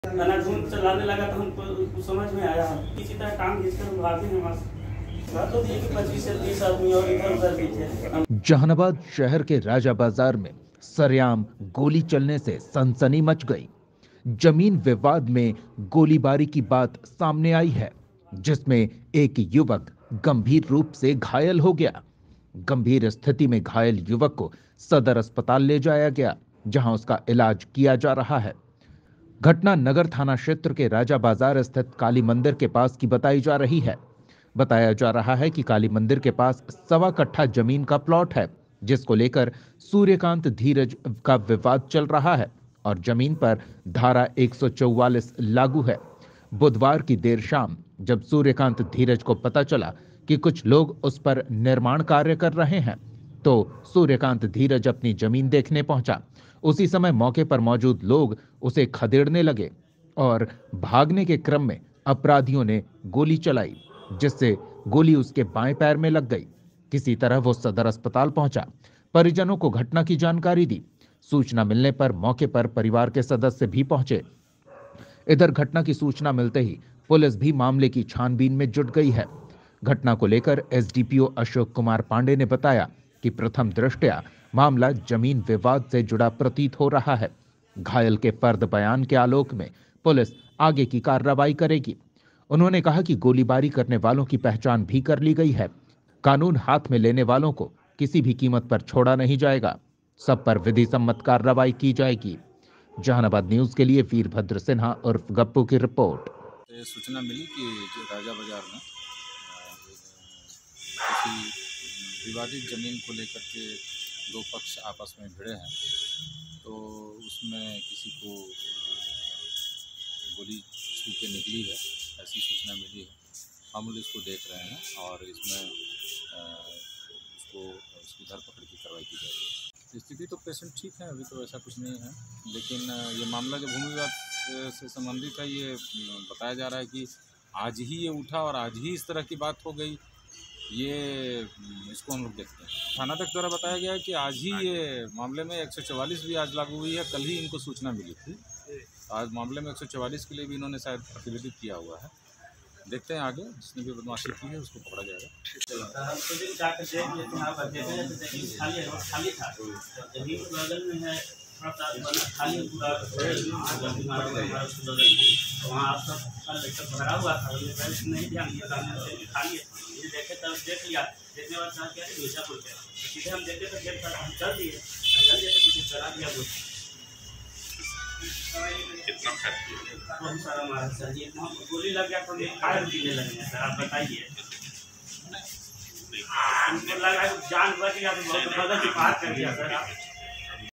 शाम में राजा बाजार में हुई ठाएँ ठाएँ, जमीन का था विवाद, एक व्यक्ति अस्पताल में भर्ती। जानिए पूरी बात
जहानाबाद समाचार//LandDispute